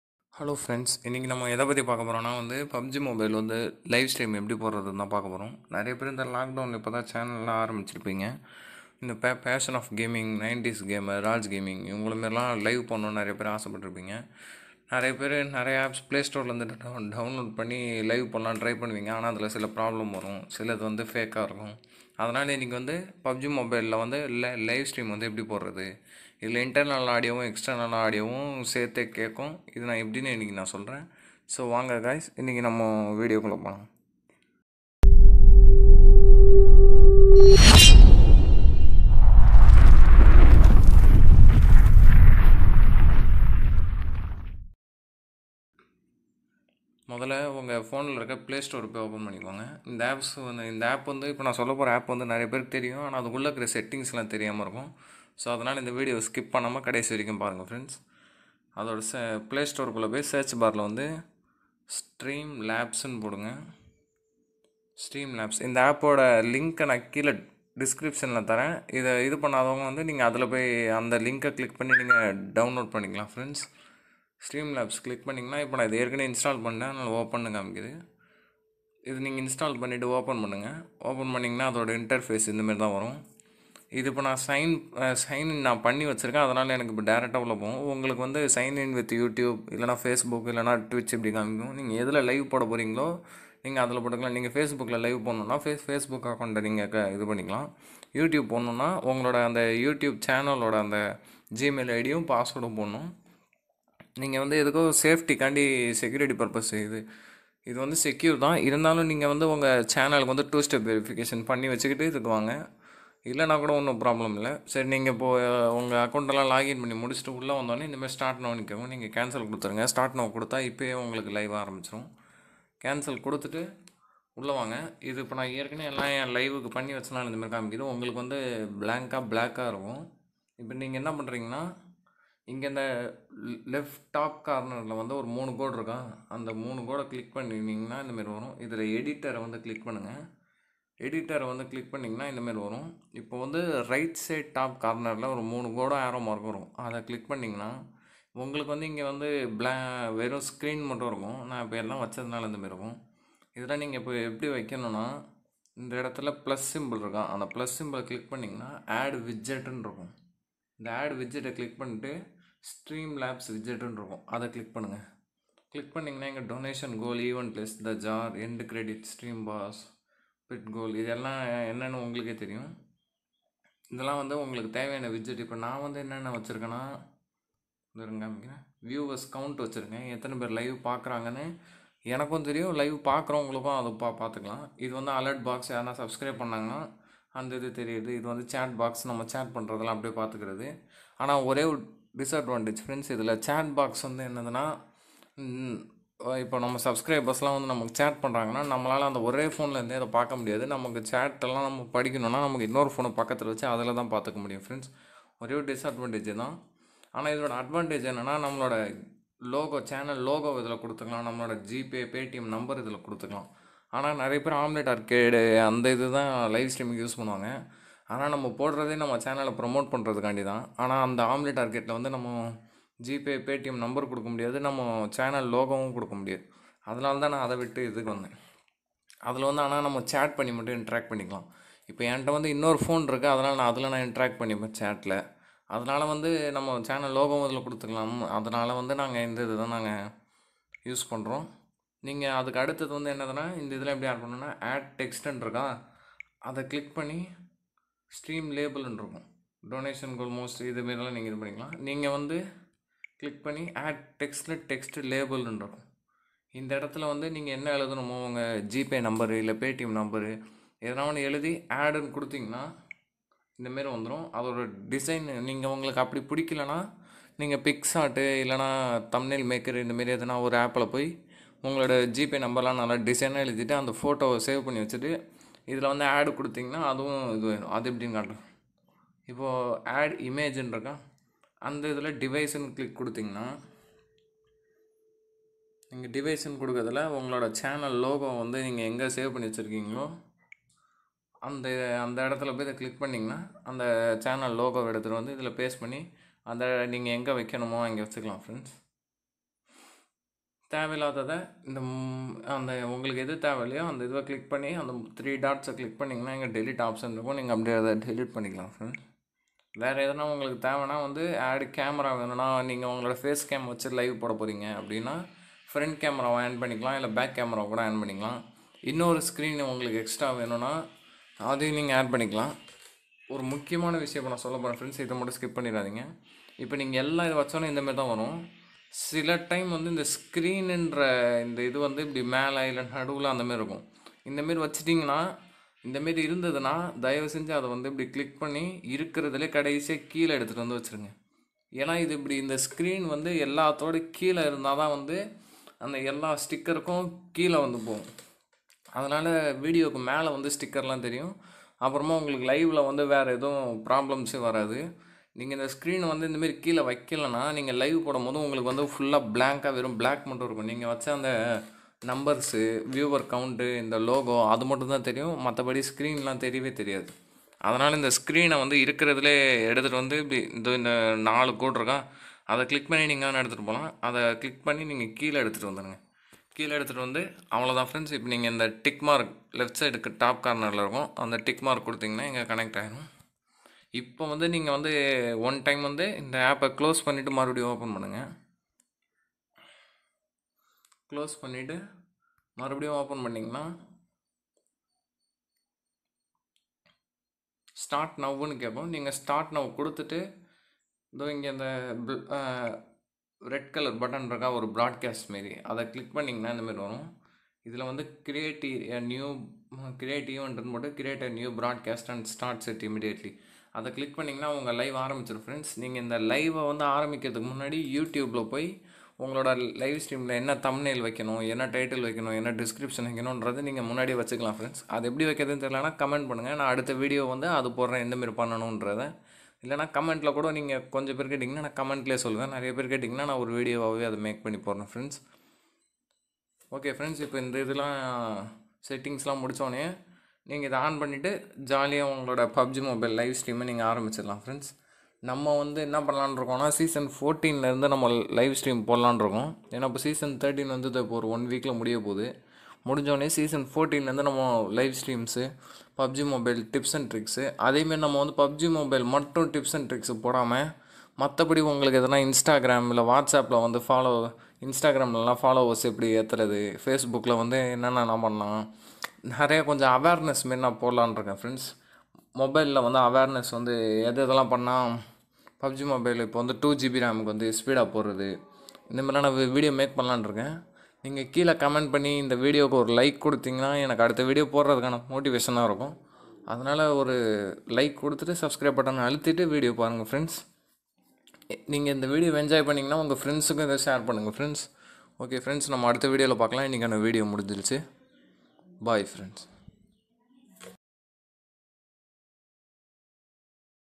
Streamlabs | How to put pubg mobile live stream on your channel
About video :

Device : vivo z1 pro
Tier : ace
App name : streamlabs

How to live stream pubg mobile in android. so guys after seeing this video you all know how to put live stream pubg mobile on android. You can live stream like raj gaming, 90s gamer,  passion of gaming. You can also put them like pubg mobile live stream and how to put pubg mobile  live stream.

So guys i hope all u enjoyed the video.